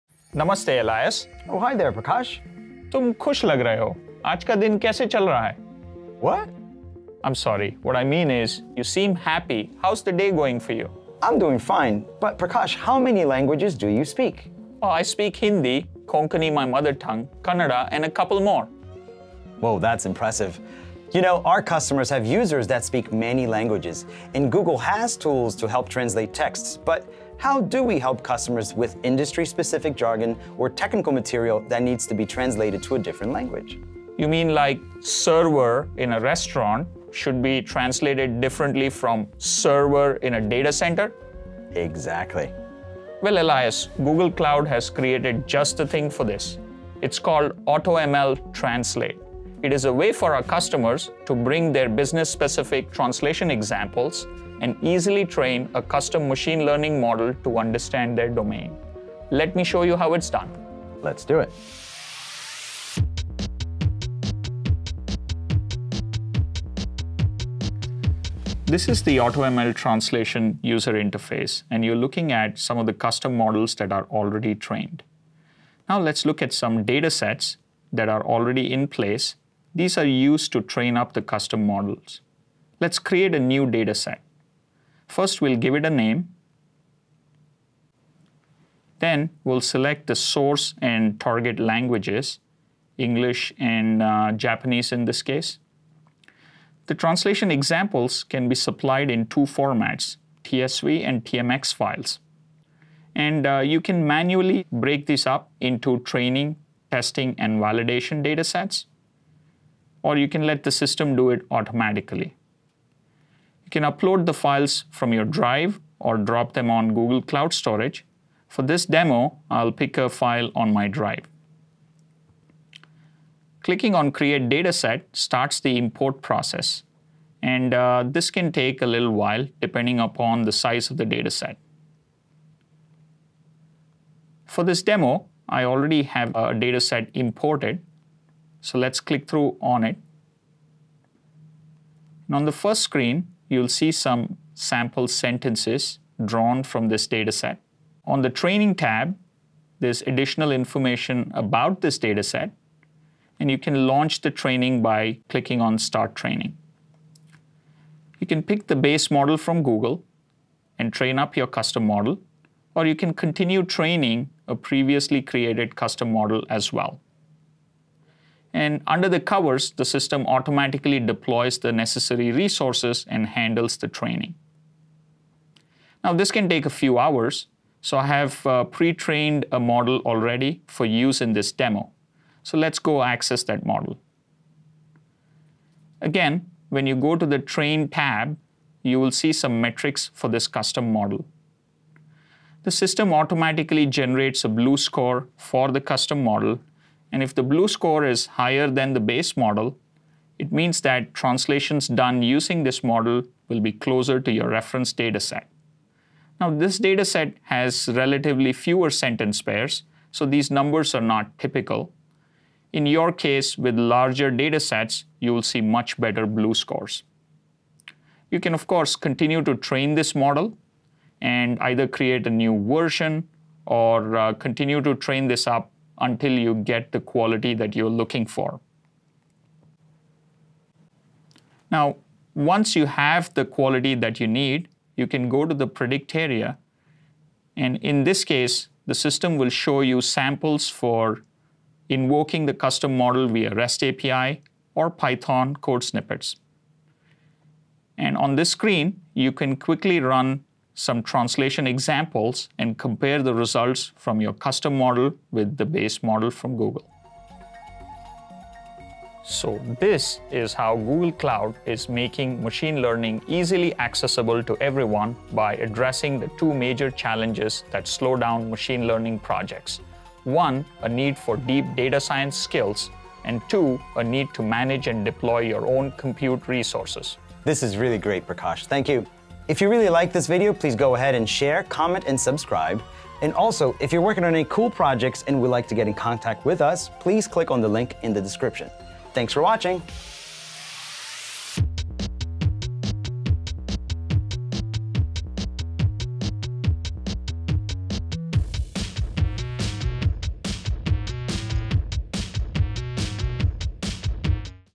Creating Custom Machine Learning Models using AutoML Translate
Google's Translation API can be an easy solution to translations but what happens when you need to create custom translations? Join Elias and Prakash as they demonstrate AutoML Translate and set up a custom translation model without writing any code!